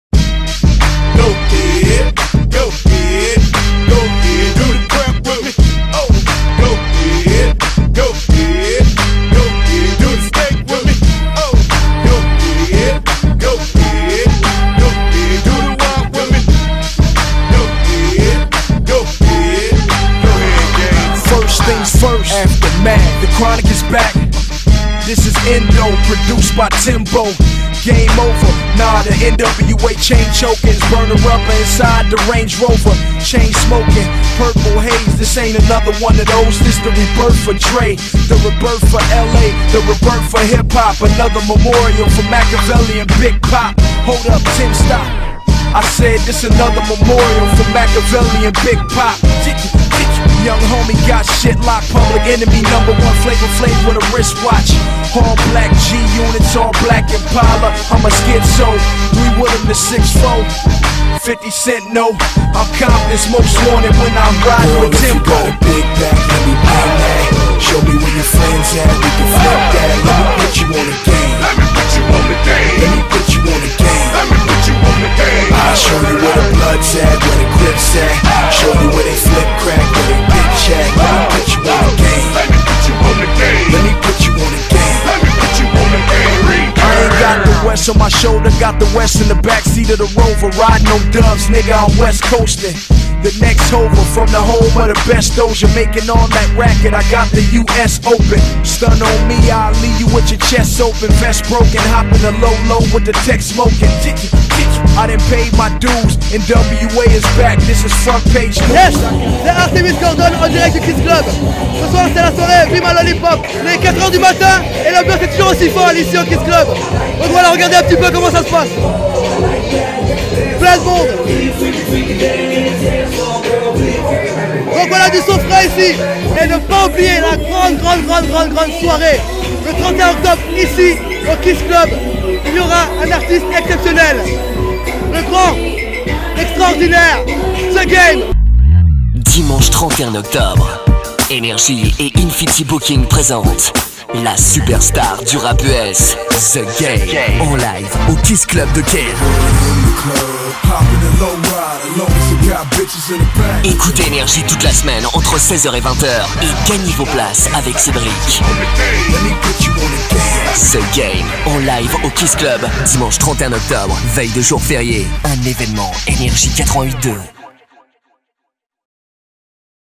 ☆ SAMEDI 23 OCTOBRE ☆ BE MY LOLLIPOP ☆ KISS CLUB ☆
KISS CLUB PRESENTE

SAMEDI 23 OCTOBRE

BE MY LOLLIPOP

Distribution de Sucettes pour les plus gourmands et surtout gourmandes afin de savourer encore d'avantage une délicieuse soirée offerte par le KISS CLUB !!!

Une soiree animee par ARTEMIS

Ainsi que DJ GHENDAPAUL & DJ ILLMATIC MIKE

Photography by JASON KISTNEN